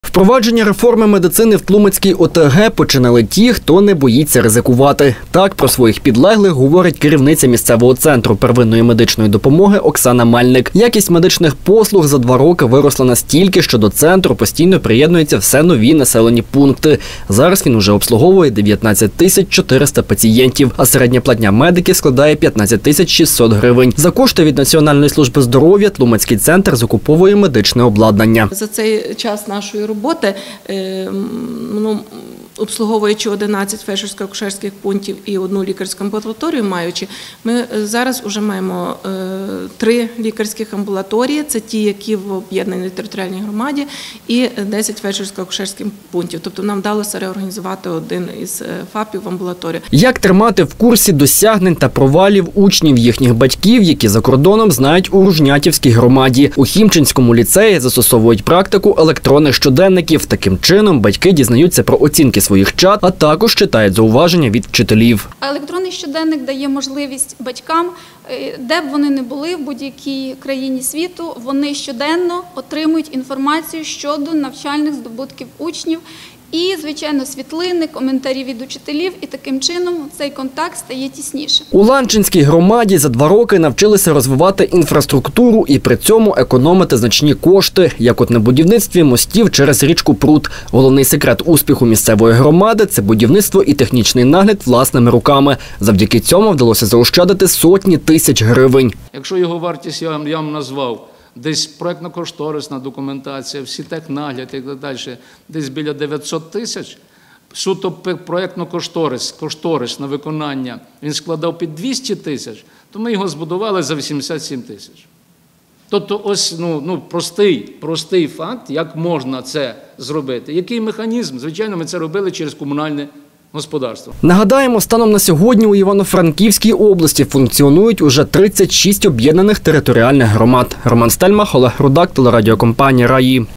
Представники прикарпатських громад розповіли про свої успішні практики
В Івано-Франківську представники прикарпатських громад розповіли про свої успіщні практики. Це і розвиток медицини, і розбудова інфраструктури, і участь в міжнародних проектах.
Переконують - за два роки функіонування реформи децентралізації мають чим похвалитися. Більше про історії громад розкажуть наші журналісти.